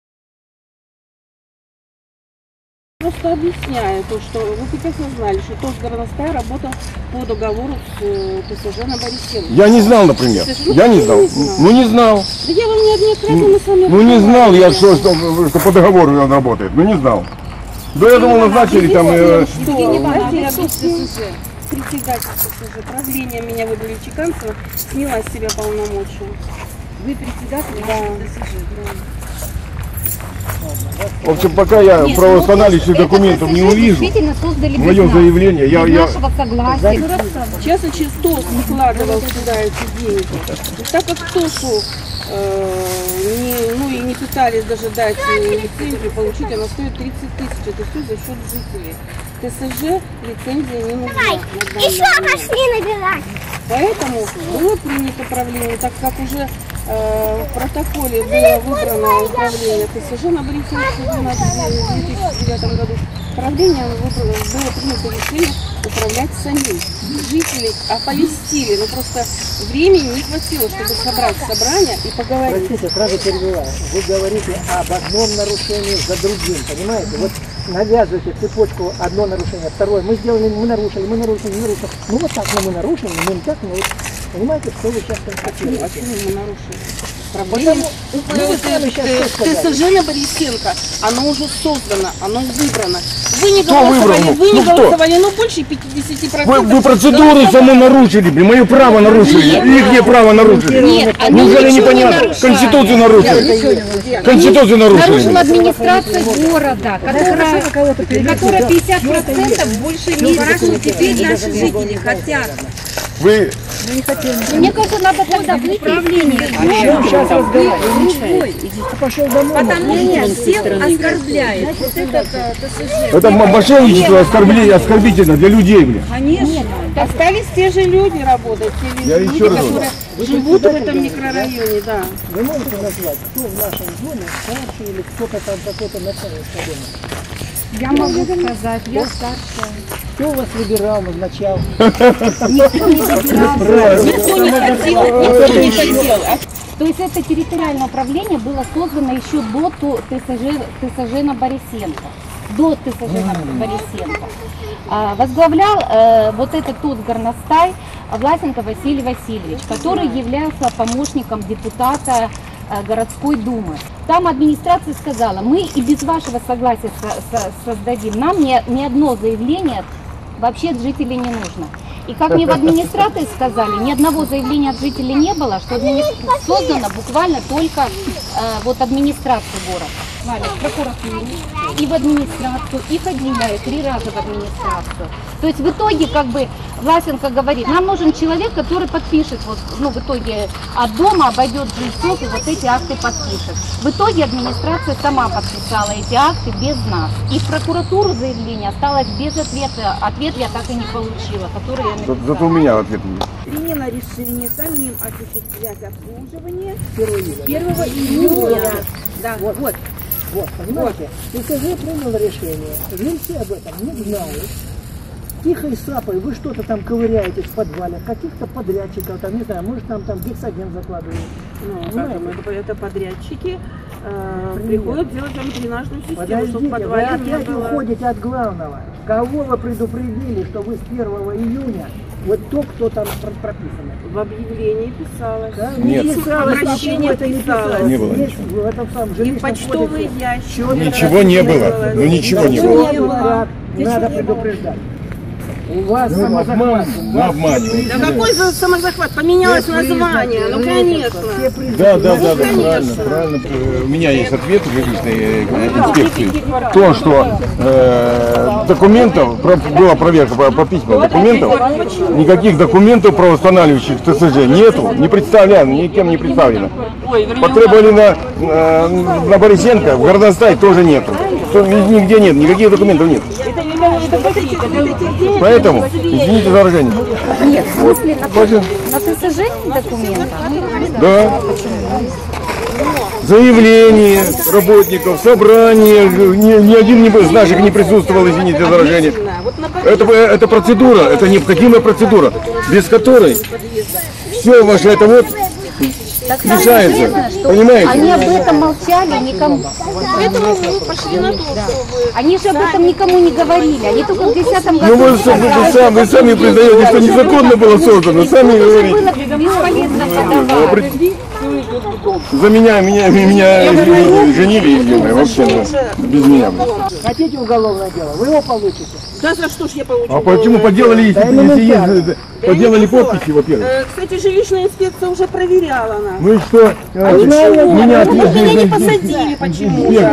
Недовольство жителей ТСЖ "На Борисенко"
Жители Владивостока считают, что ТСЖ  было создано незаконно.